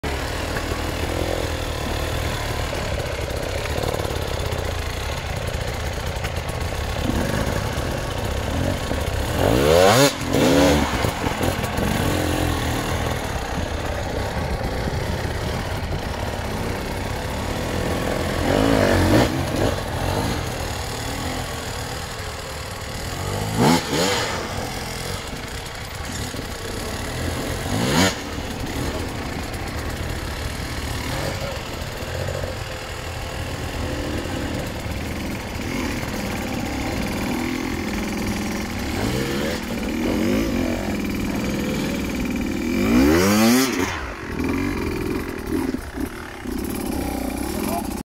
Graham Jarvis în Action, short Technik climb, practice program
ENduro Training day with the KING of Enduro - Graham Jarvis
enduro shorts
dirt bike short